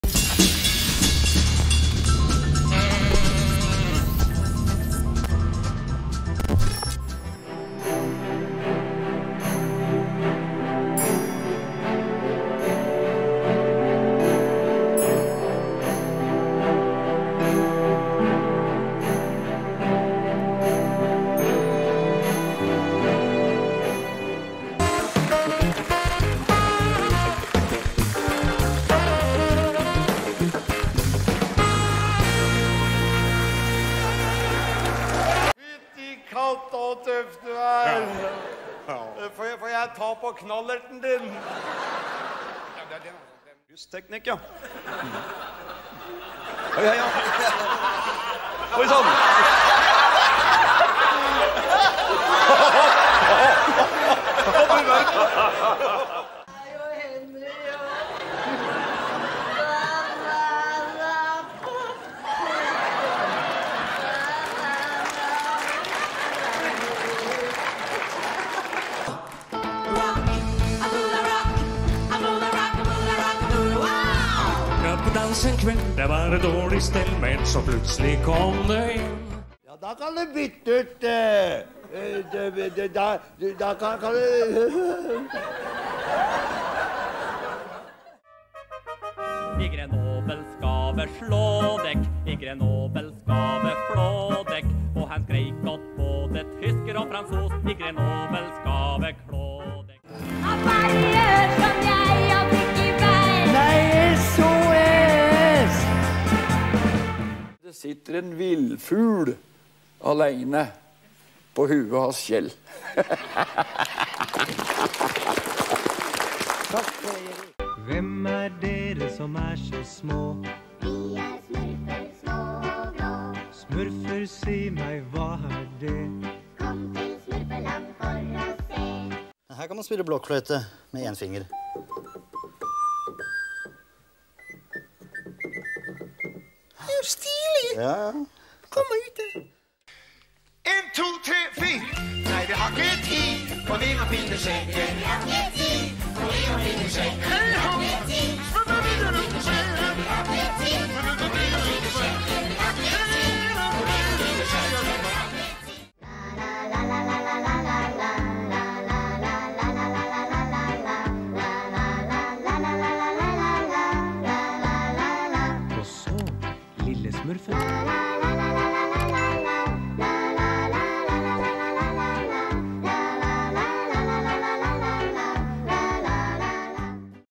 Mimre-TV Trailer 2023
Mimre-TV Trailer 2023. We are a retro nostalgia channel, who post old vintage retro stuff.
40% International content and 60% Norwegian content. Norwegian Shows are tagged with [NOR] in the titles.

any violated content if requested. THANKS!